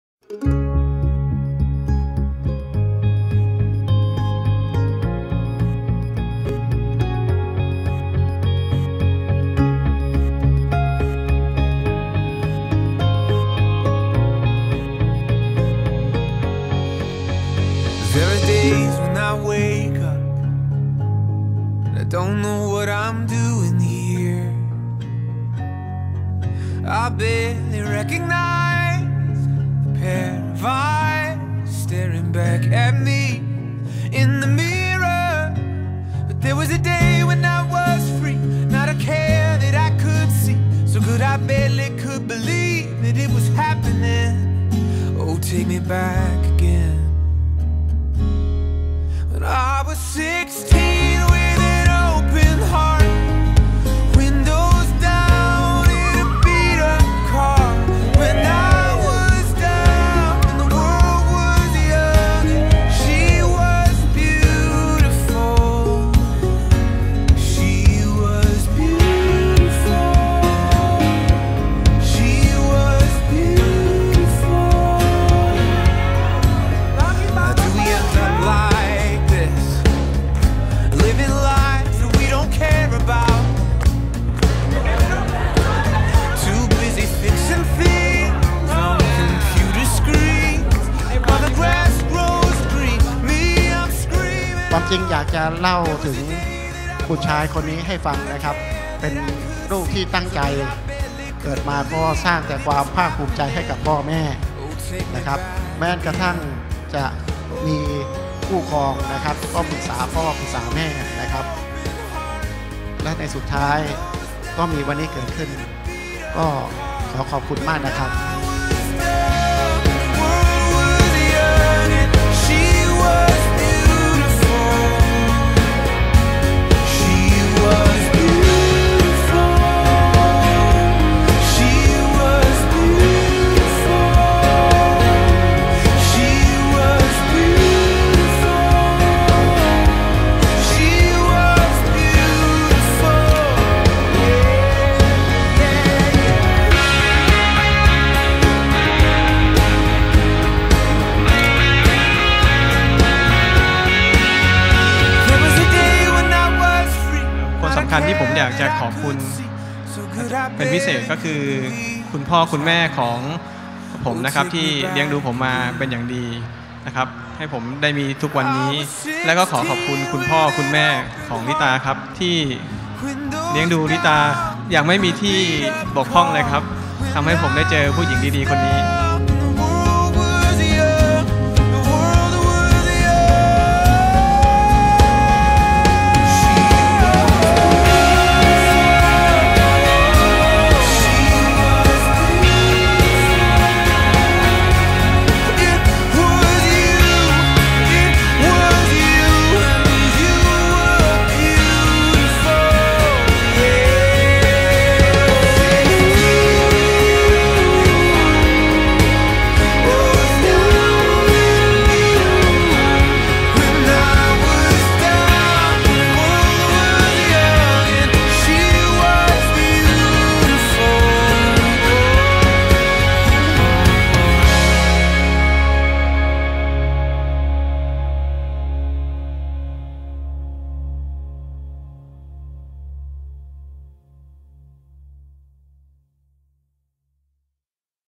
LITA & M // Trailer
ประมวณภาพวินาทีแห่งความประทับใจ
ของสองคู่บ่าวสาว คุณลิตาและคุณเอ็ม
ที่โรงแรม ไดมอนพลาซ่า สุราษฎร์ธานี

มีความสุขมาก ๆ นะครับ
CHEERS!!